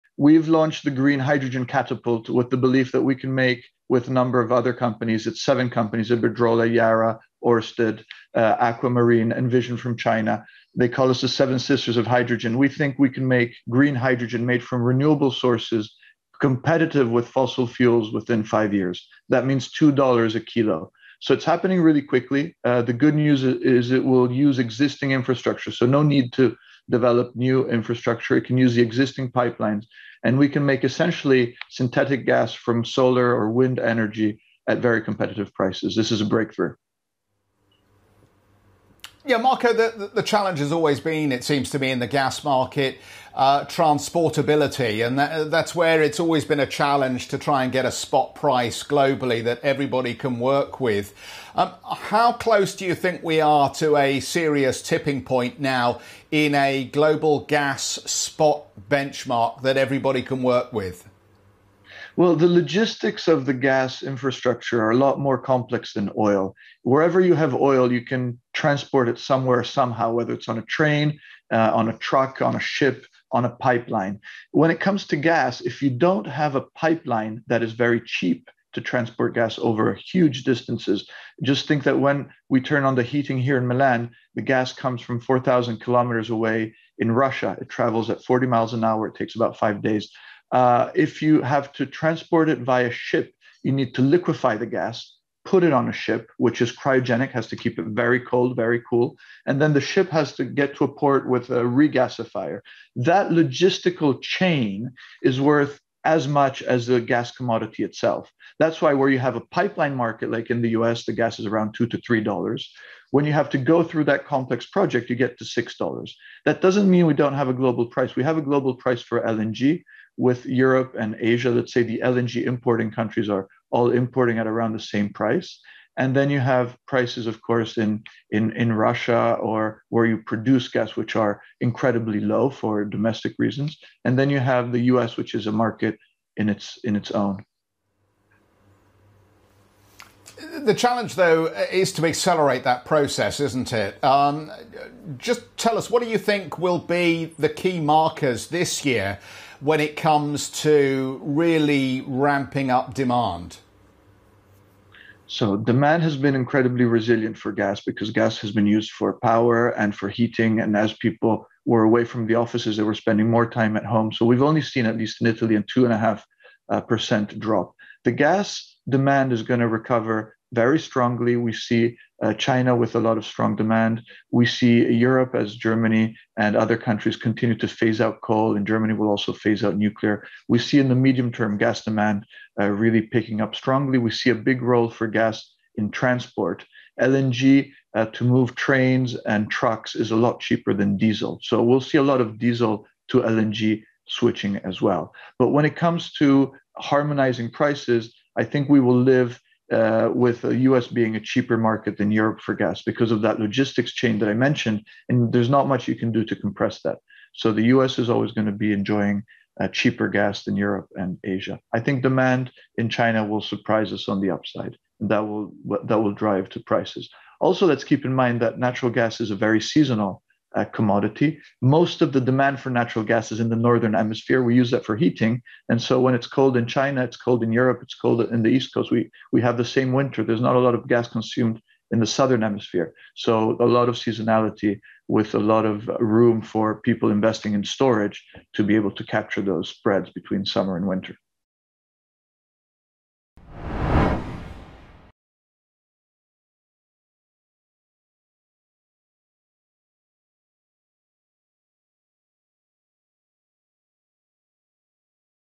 Green hydrogen can compete with fossil fuels within 5 years, SNAM CEO says
Marco Alverà, CEO at SNAM, discusses the Italian's company's plans around energy transition.